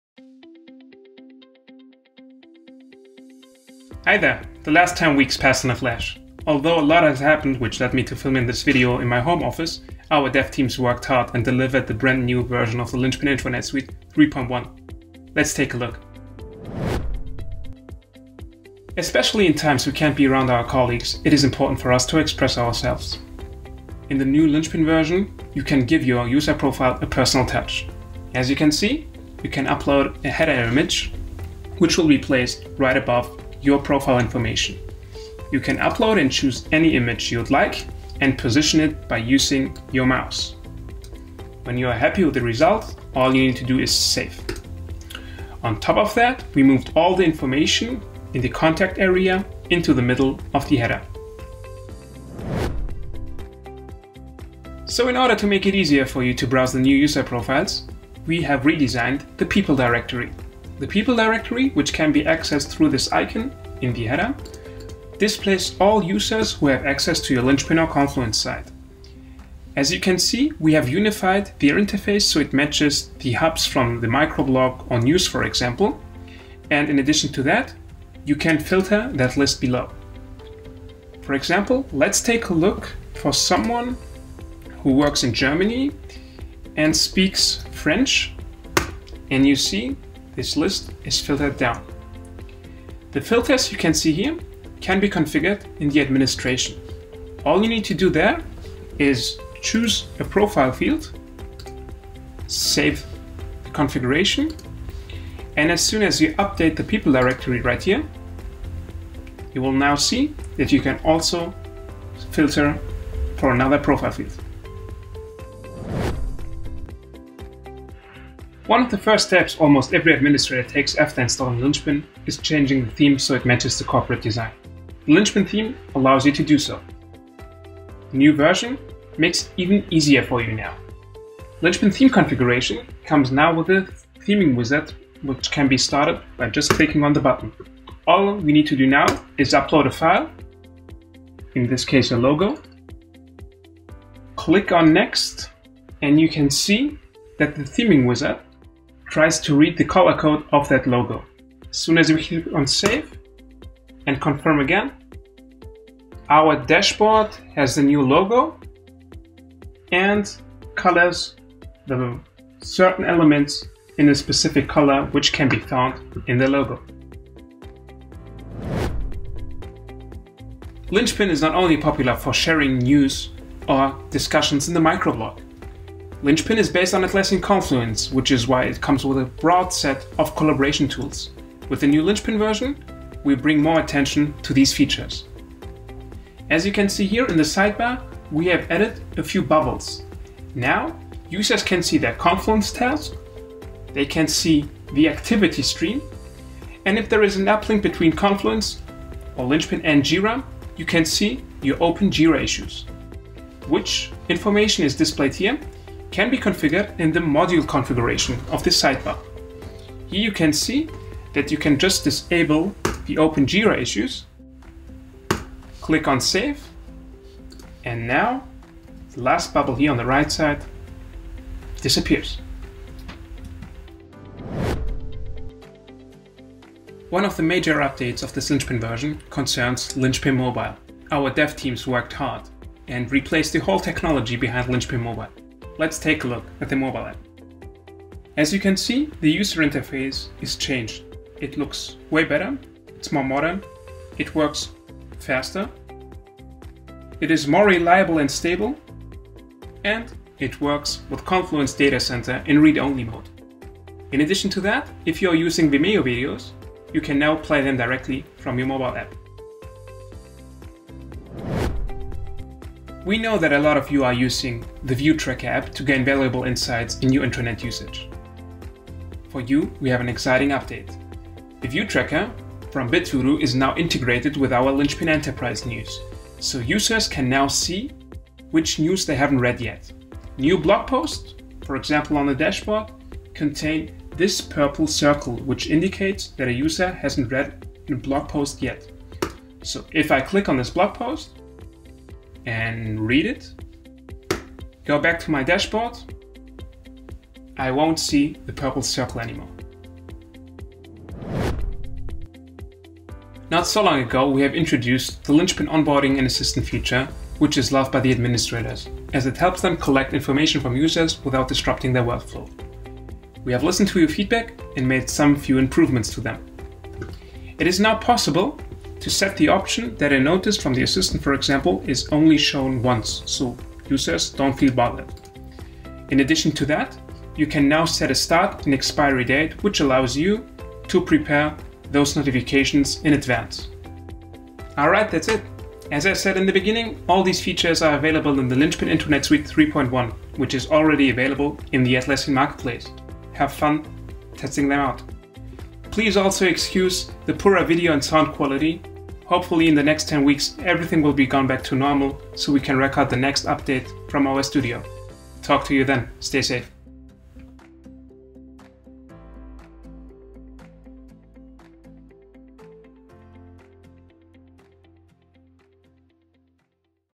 Linchpin Suite Release 3.1 – What's new?
Hi, I'm Adil and I am very happy to present the new feature release for the Linchpin Intranet Suite. In this video, I’ll provide you with a quick overview of the most important changes and improvements we have made in this update.

0:03 - Introduction
0:16 - Shiny user profiles
0:52 - Enhanced people directory
1:56 - Theming Wizard
2:48 - Sidebar improvements
3:45 - Mobile app with a completely new technical foundation
4:20 - News integration with Bitvoodos View Tracker for Confluence
5:01 - Improvements for the assistant / onboarding experience

Linchpin is an intranet where you can find your feet instantly – a visually familiar tool following your company’s look and feel, with content and features tailored to you, your work, your interests and your profile: News, events, announcements, information for your location and more.

//SEIBERT/MEDIA